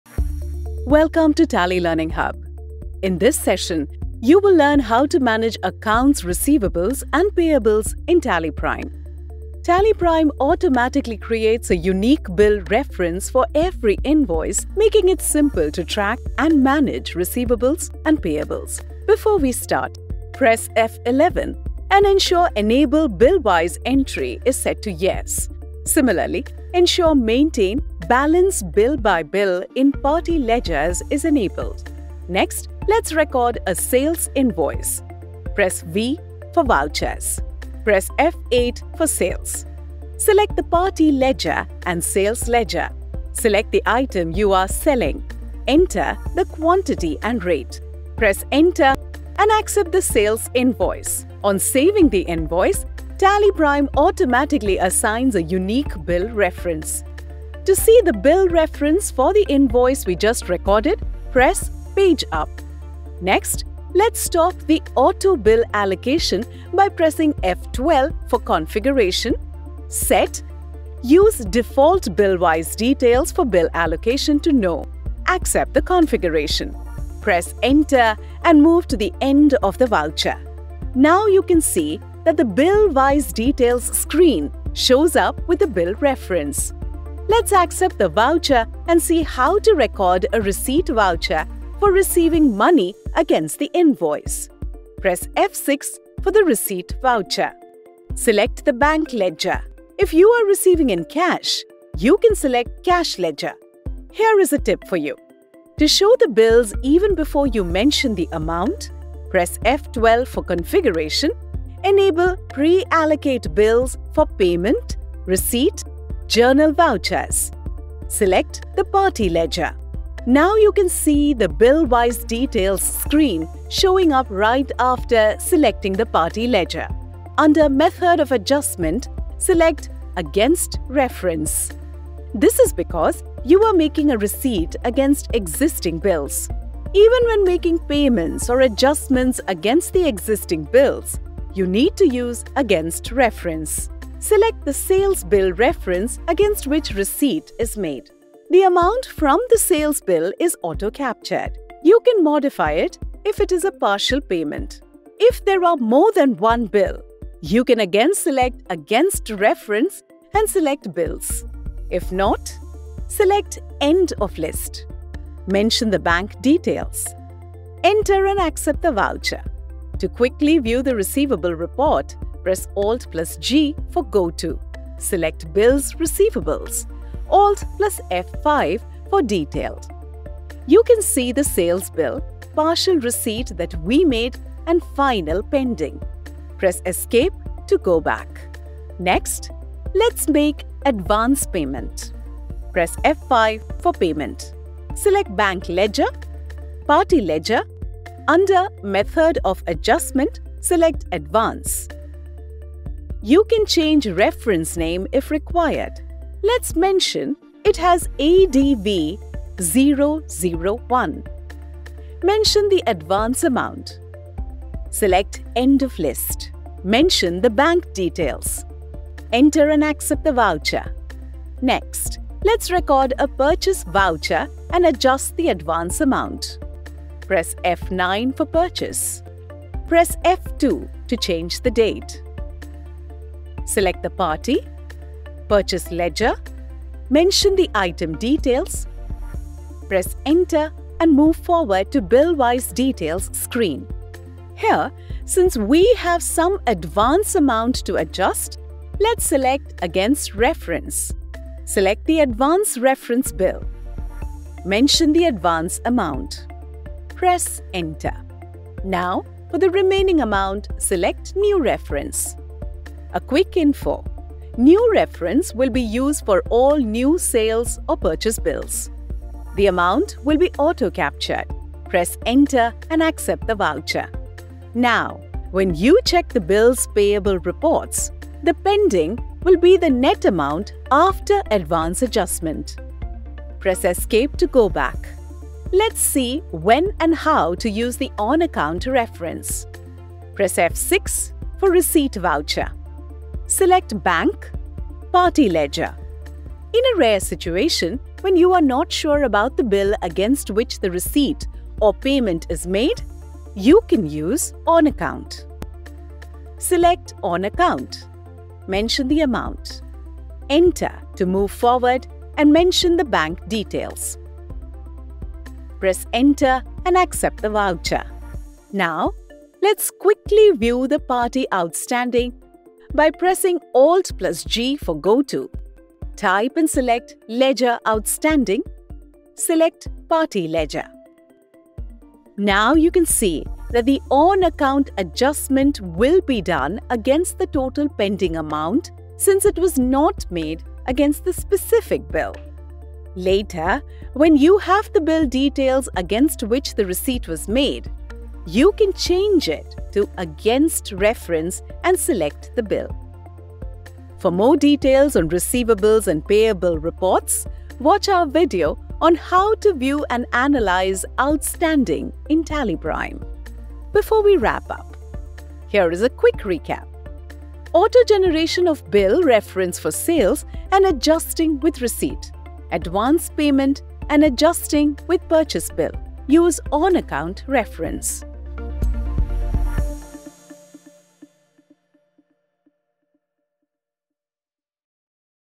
How to Manage Receivables and Payables using Billwise Details in TallyPrime | Tally Learning Hub
In this Tally Learning Hub video, you will learn how to manage accounts receivables and payables using bill wise details in TallyPrime.